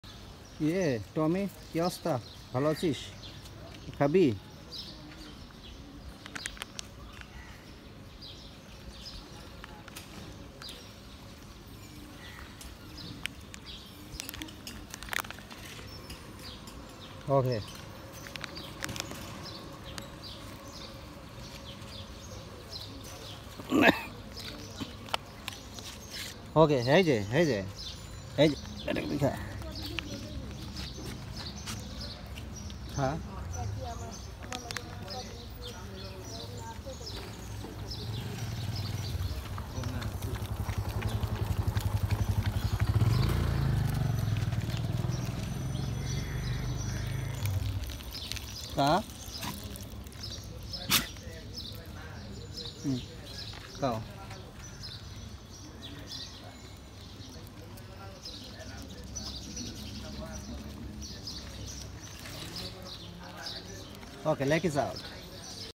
9 April, 2023 আজ ধলু কালু জন্মদিন দুধ রুটি খাবে ! Dog Happy !Gift By Shohag
9 April, 2023 আজ ধলু কালু জন্মদিন দুধ রুটি খাবে ! Dog Happy !Gift By Shohag

আসুন বন্ধুরা প্রতিদিন একটি প্রাণীর মুখের হাসি ফোটায়।😻